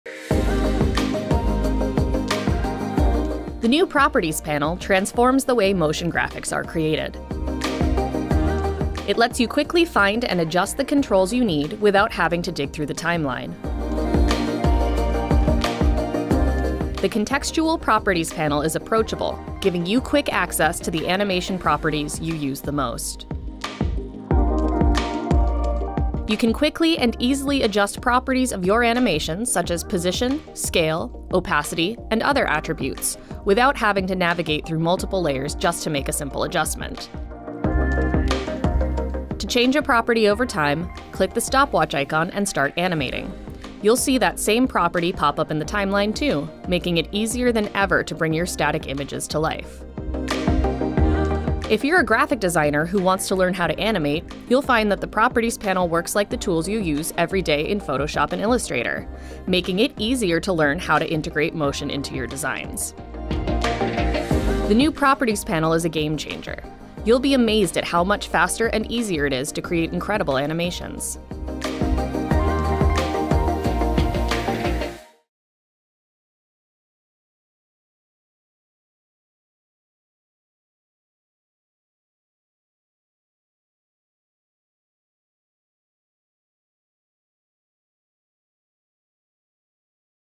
Introducing Properties Panel in After Effects - Effortlessly Create Motion Graphics | Adobe Video
Level up your motion graphics game with the enhanced Properties Panel in AfterEffects. Say goodbye to unnecessary twirling and hello to quick access to your most important properties. Whether you're a seasoned pro or a beginner transitioning from Photoshop and Illustrator, you'll feel right at home with familiar controls in convenient locations.